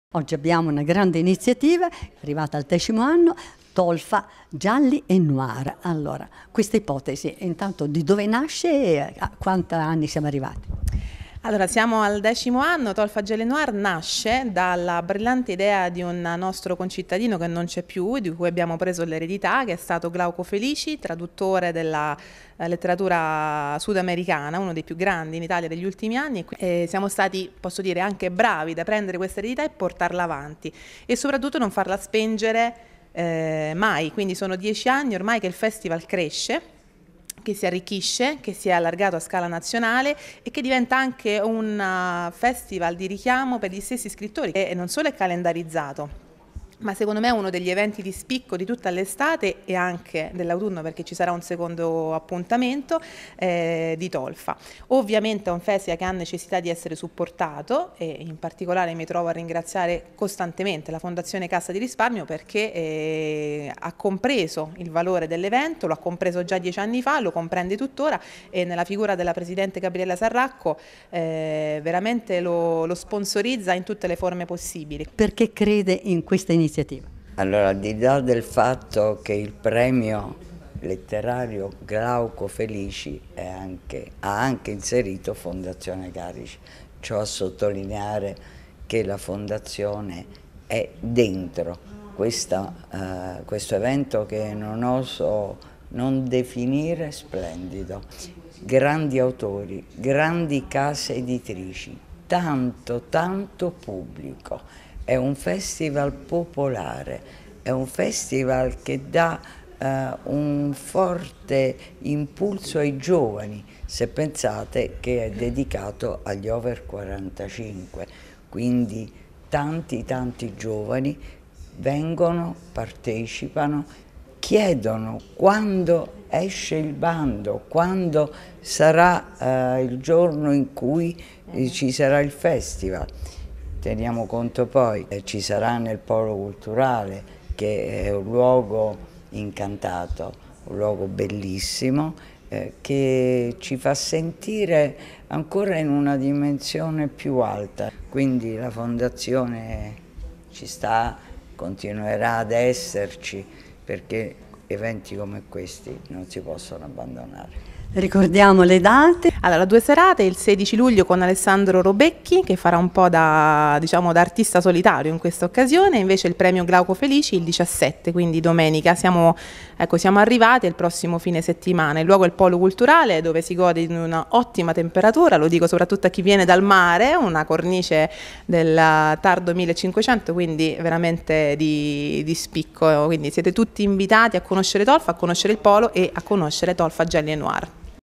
A Tolfa una doppia serata per la decima edizione del "Gialli & Noir"
Un doppio appuntamento per festeggiare il decimo anniversario del Festival Tolfa Gialli & Noir. Al Polo Culturale di Tolfa protagonista della prima serata di sabato, dalle ore 21.30, sarà il giornalista, autore televisivo, critico musicale e conduttore radiofonico Alessandro Robecchi.